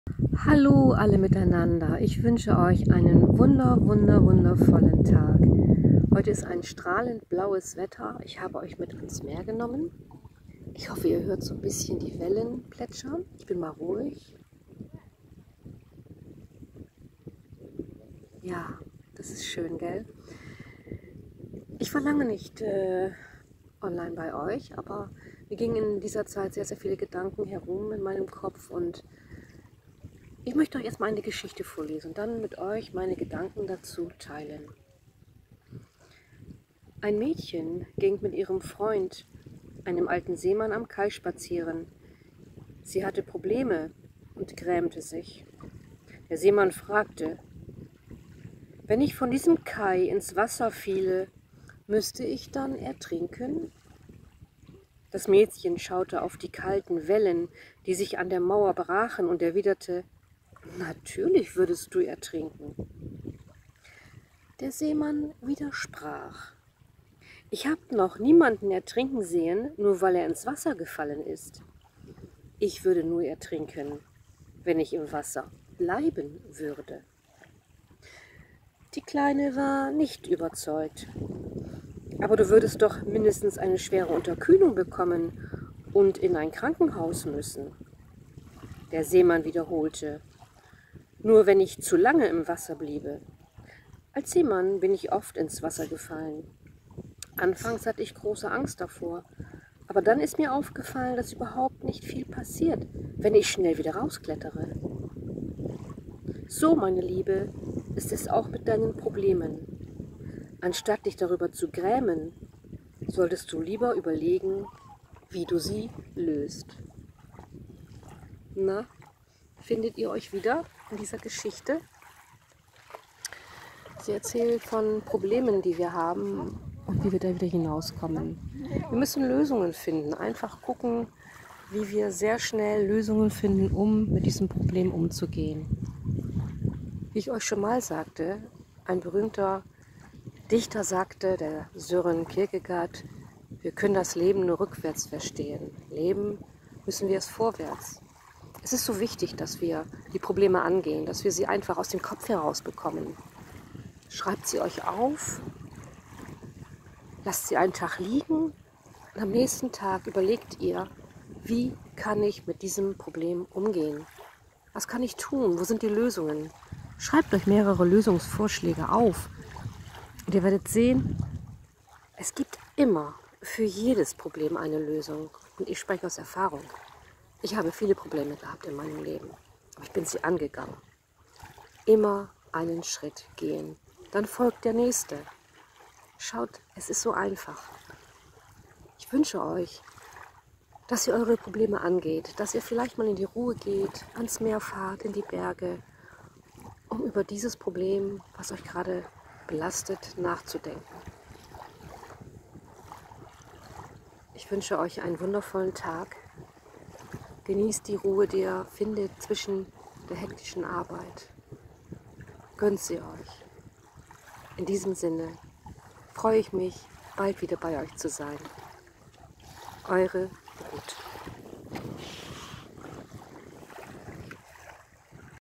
iss smart - Probleme lösen! Aber wie?
Wer kennt es nicht: Sobald ein Problem auftaucht, gehen wir ihm aus dem Weg. Warum machen wir das? Und noch viel wichtiger: Wie können wir es besser machen? Probleme begegnen uns überall. Sie betreffen unseren Job, die Gesundheit und das Privatleben.

Probleme haben also gar nichts Negatives an sich. Es sind Aufgaben, die zum Leben einfach dazu gehören. Wichtig ist nur; dass wir schnell an die Lösung arbeiten. Sonst fressen uns unsere Probleme auf!